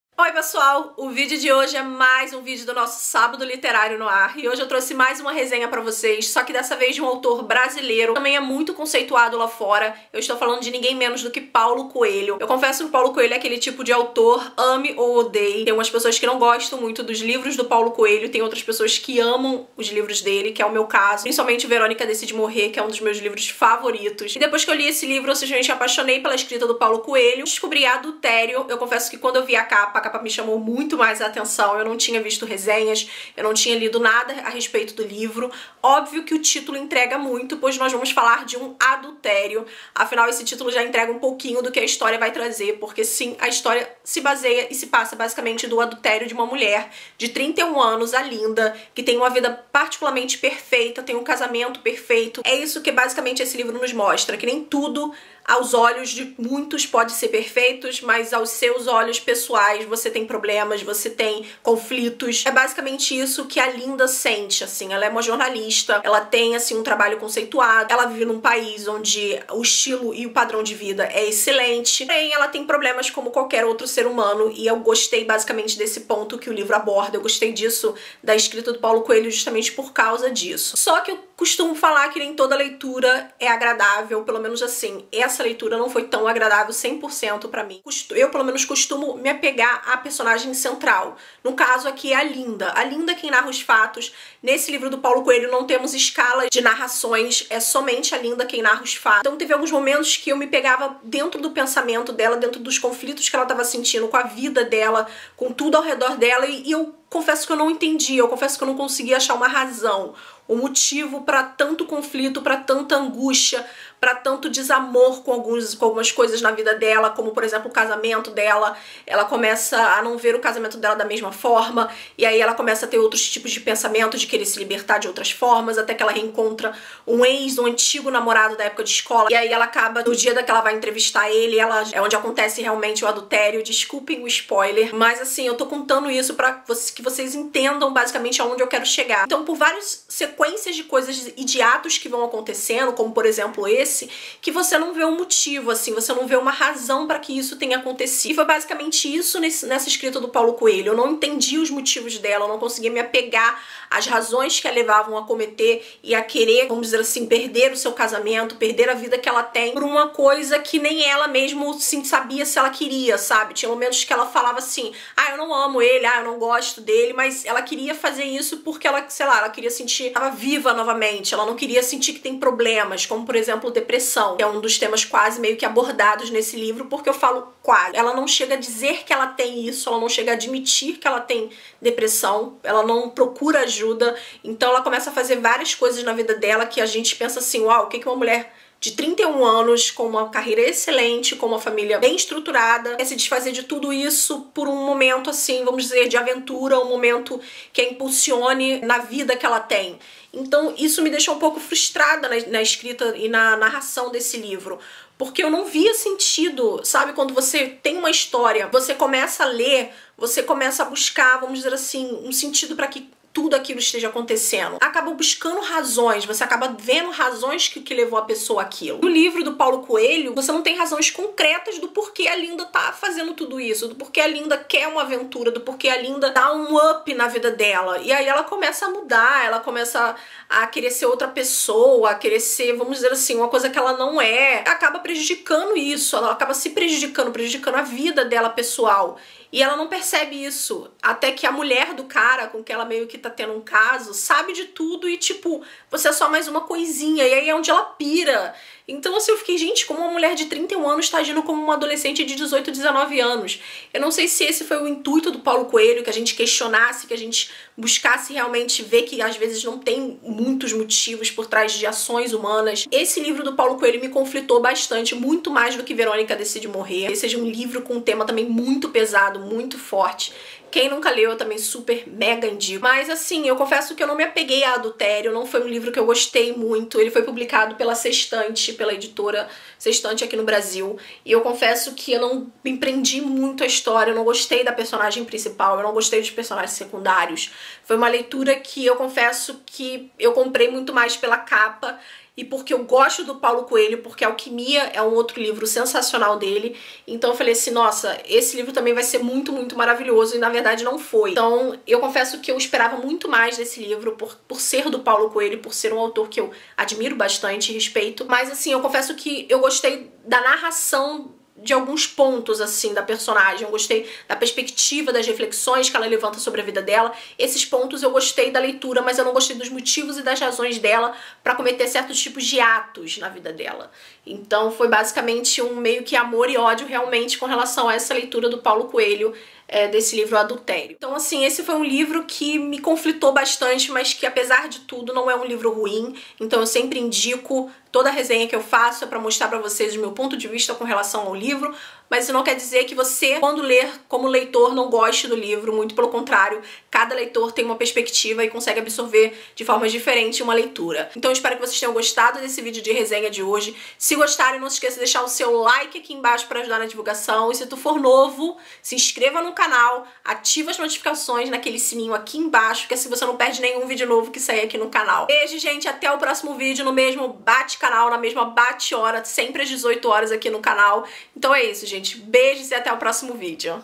ADULTERIO - PAULO COELHO | RESENHA
Vídeo de resenha para o nosso Sábado Literário do livro "Adultério" do autor Brasileiro Paulo Coelho que foi lançado pela editora Sextante!

I N S C R E V A - S E N O C A N A L !

♡ VÍDEOS NOVOS: Segunda & Quinta ás 19h30 + Vídeos Extras aos Sábados